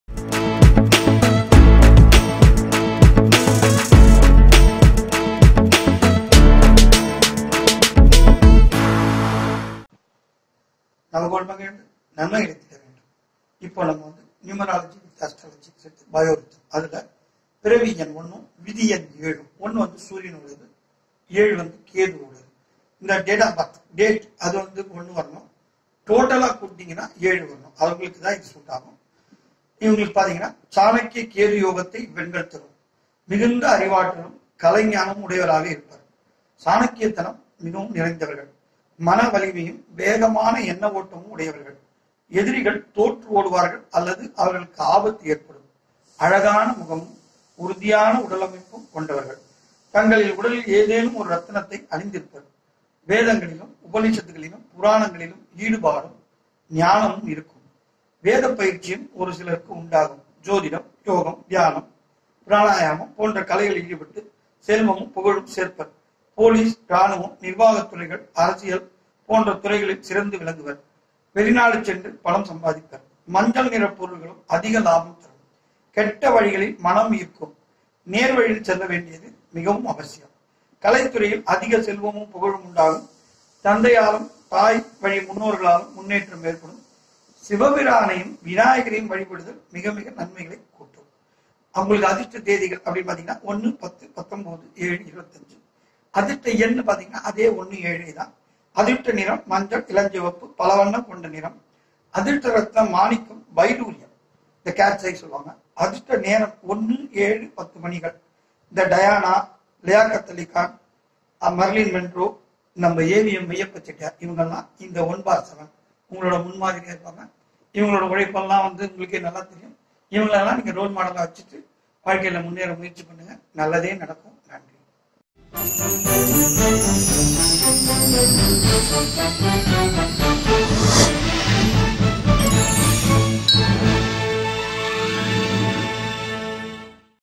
Number 1/7 Numerology Astrological Benefits | எண் கணித ஜோதிடப் பலன்கள் : 1/7
PANDIT SHREE ASOK ANNA has given the significance about Numerology in
 BIO-RYTHM (Astro science) method. In this  video he has given information about numerological numbers from (1 to 9 Birth & Fate)
Panditji has explained the uniqueness ,special features and Lucky numbers, Dates , Stones, Colors &  Also Celebrities.